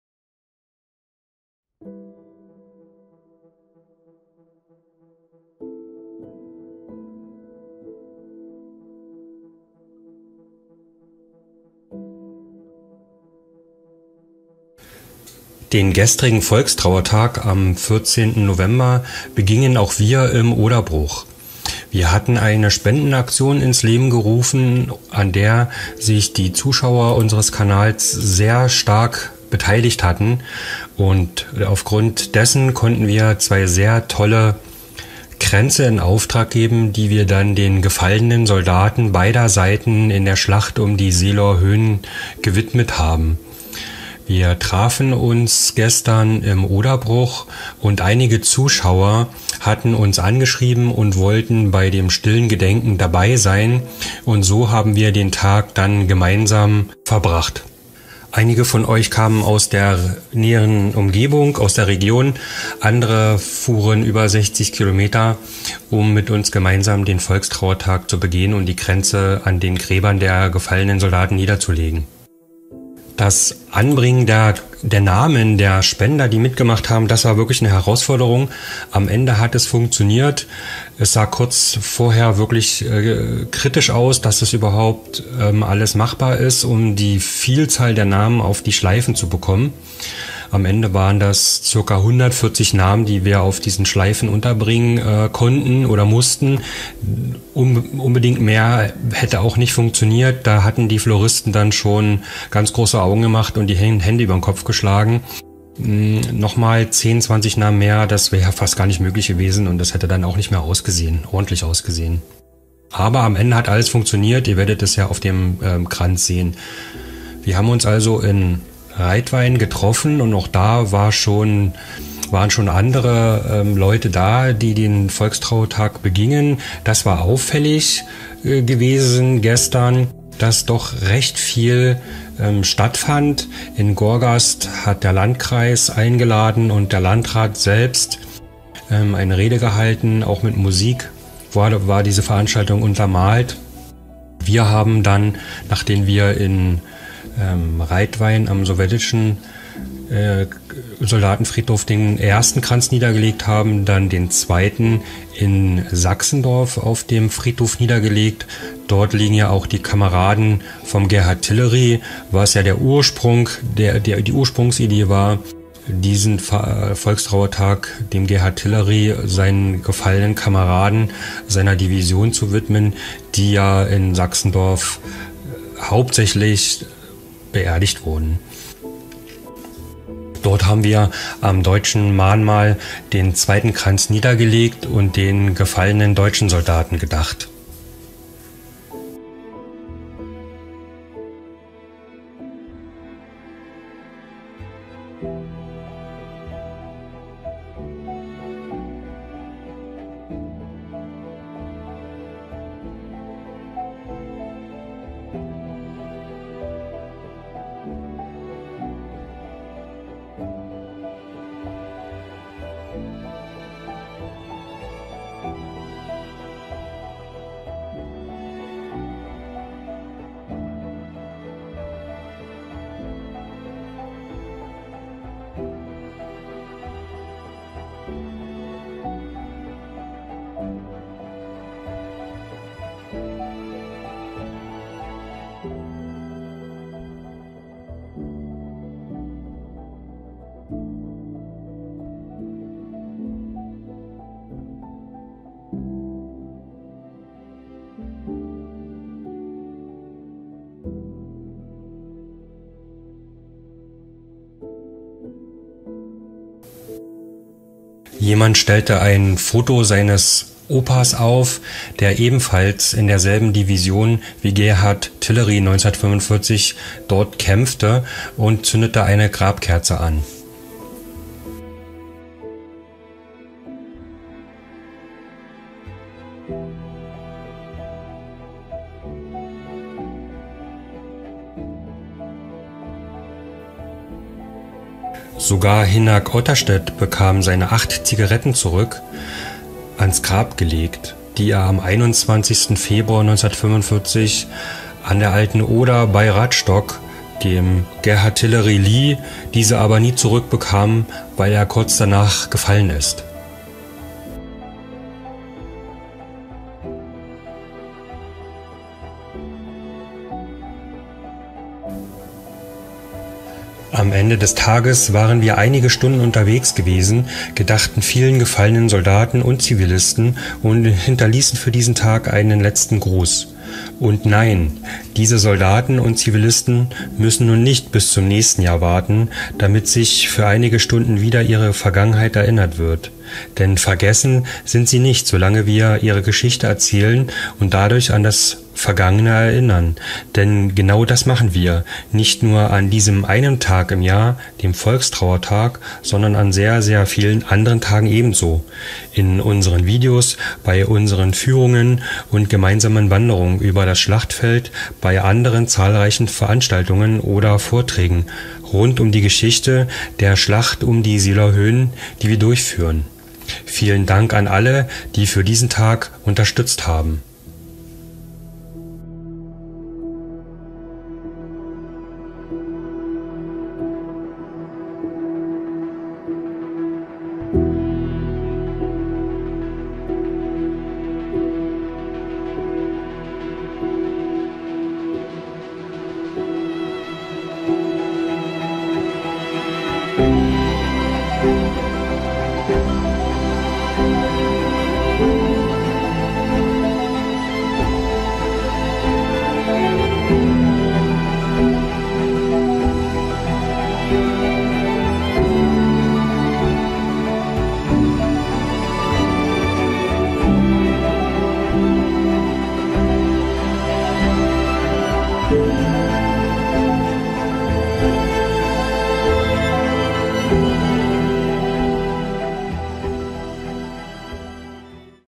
Ich trat vor ein Soldatengrab und sprach zur Erde tief hinab.......KranzSpendenAktion Volkstrauertag
Stilles Gedenken zum Volkstrauertag 2021 im Oderbruch. Zusammen mit Zuschauern, Spendern , Freunden, Mitgliedern und Mitglieder der Community begangen wir am gestrigen Volkstrauertag in Reitwein, Sachsendorf und Gorgast.

Soldatengrab

Hier schweige der Haß, denn alle die
Da unten ruhen, ob Freund, ob Feind,
Liebten die Heimat und haben für sie
Des Herzens blutigste Tränen geweint.
Für jeden trauert ein fernes Weib,
Ringt durch die Nächte sich weh und wund
Und heiligt den toten Soldatenleib
In einem stillen, erdigen Grund.